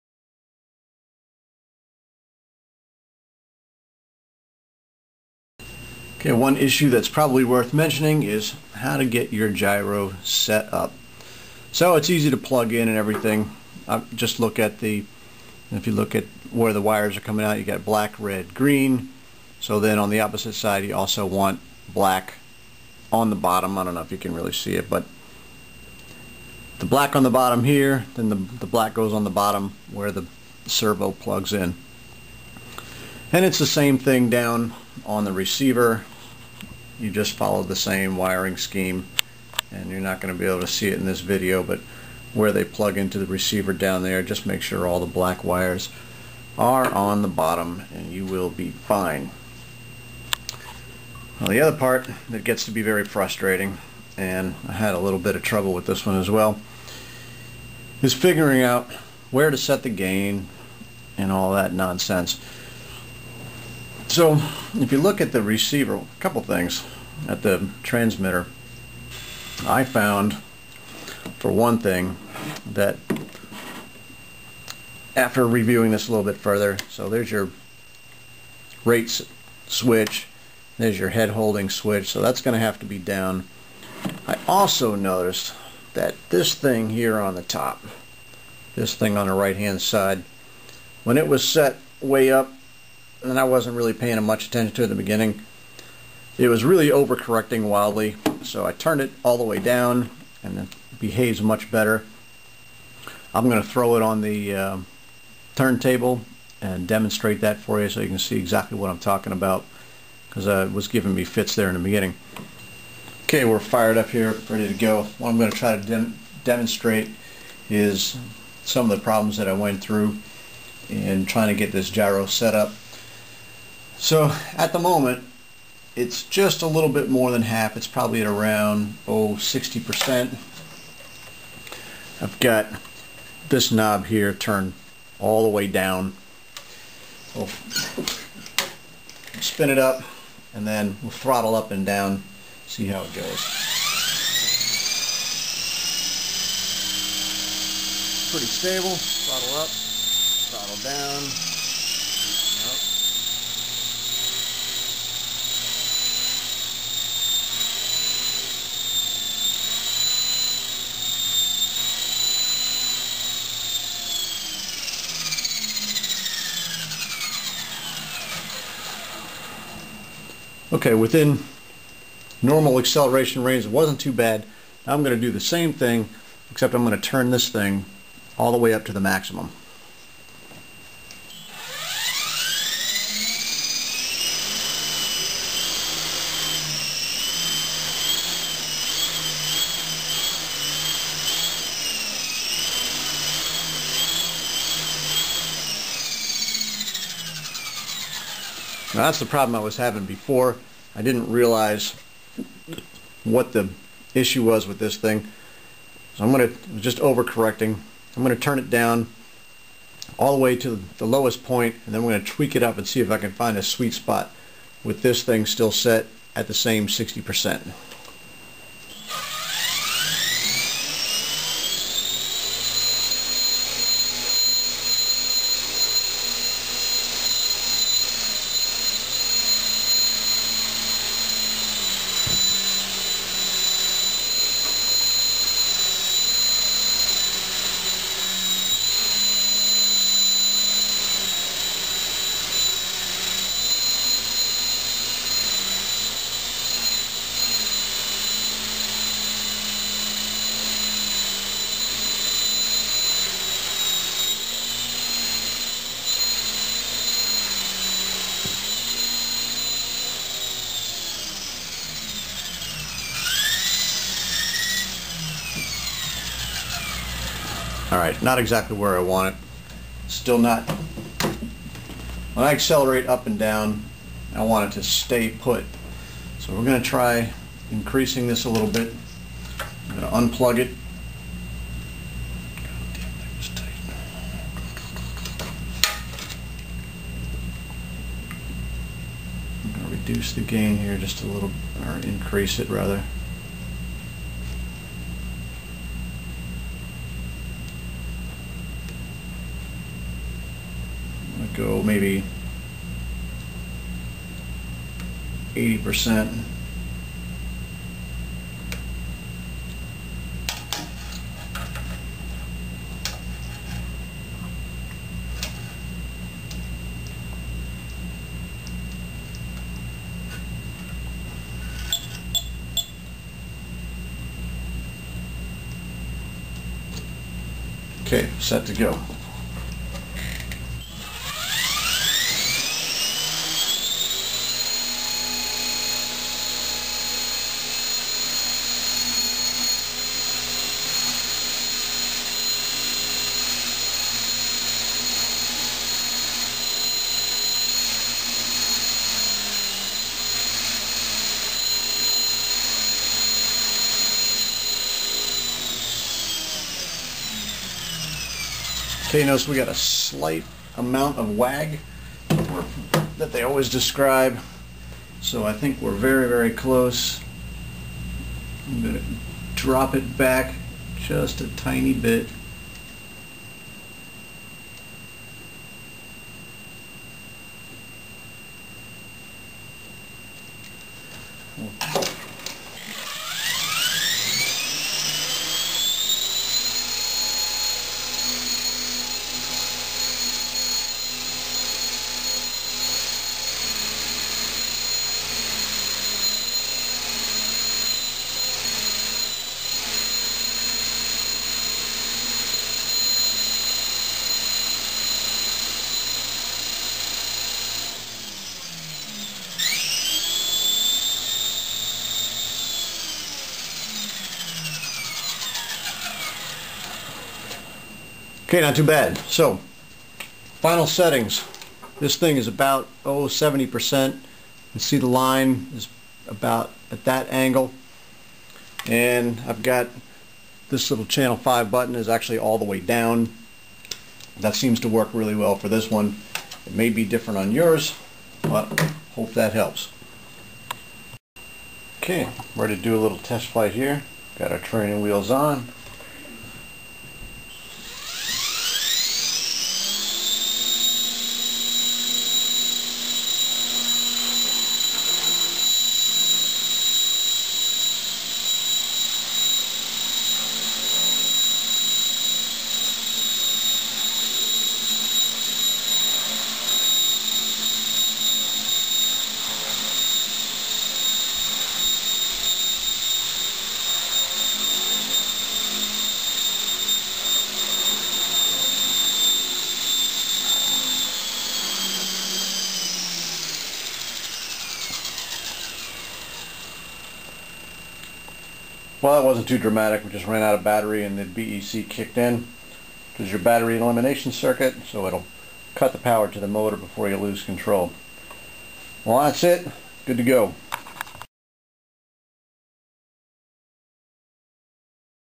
Gyro Setup on Honey Bee King V2
Gyro gain settings and transmitter settings. Turntable testing and quick test flight